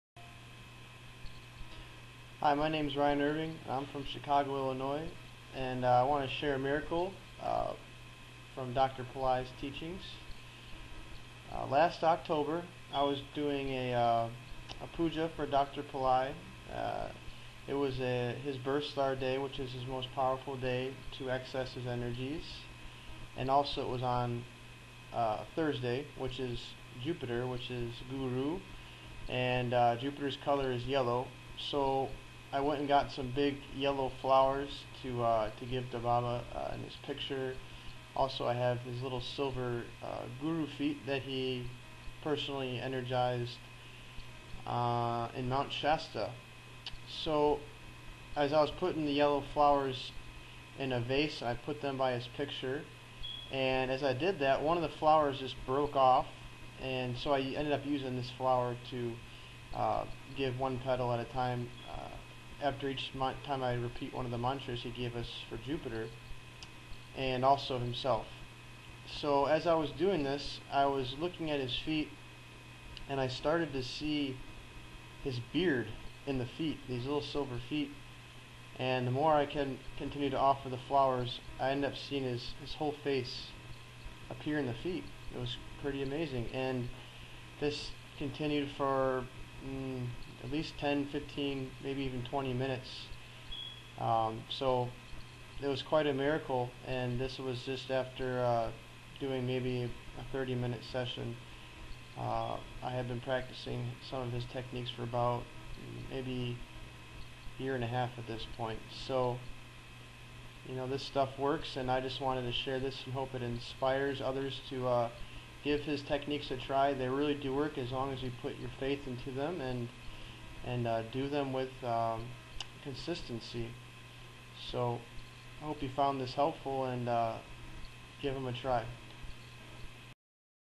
Operation Inspiration: Dr. Pillai's Face Appeared In Front of Me
Can you do me a favor? Your input would help me out

While doing a meditation, the face of Dr. Pillai appeared clear as day.